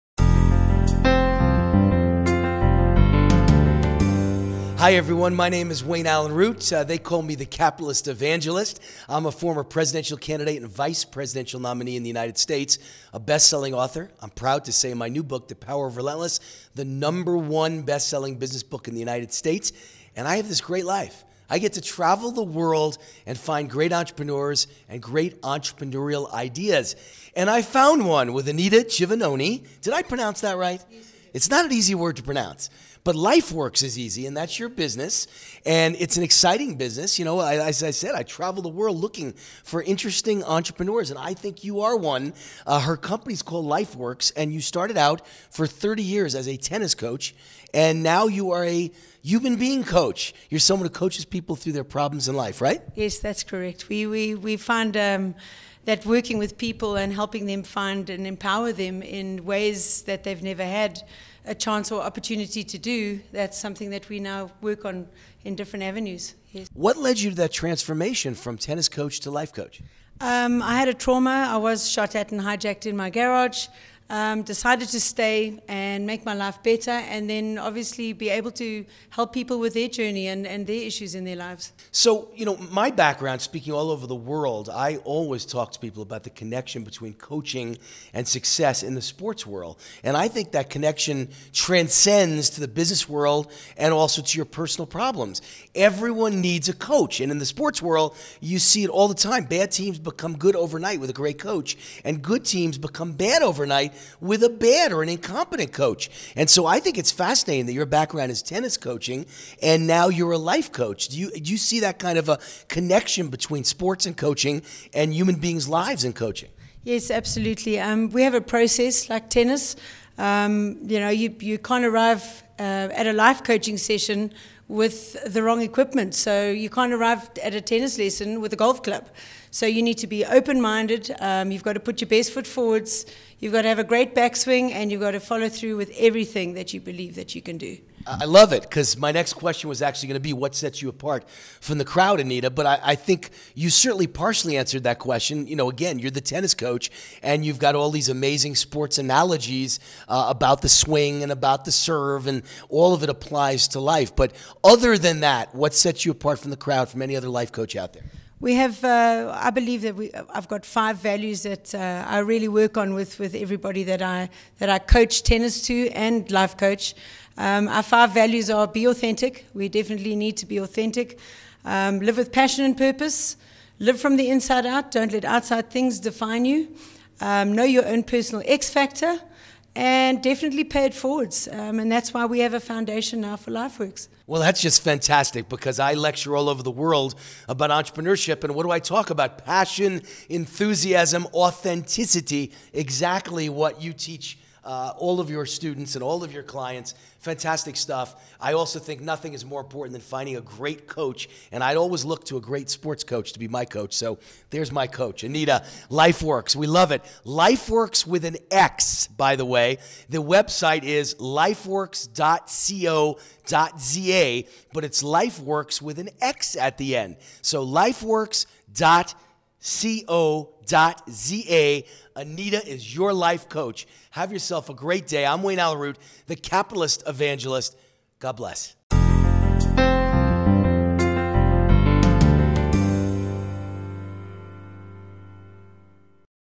Wayne Allyn Root - LifeworX
Wayne Allyn Root interviews Anita from LifeworX.co.za about her business and the link between sport and life coaching.